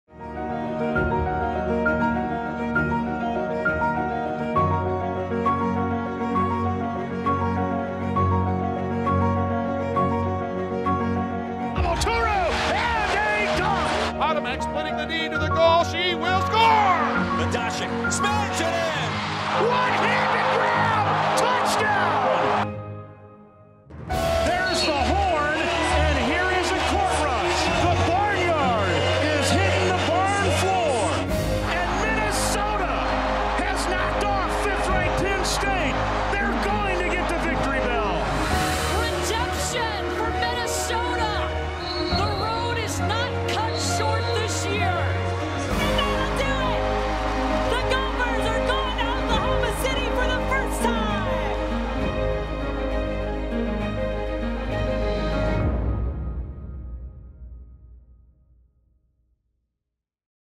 2020 Golden Goldy's Intro Video
Watch the intro video for the 2020 Golden Goldy student-athlete award ceremony.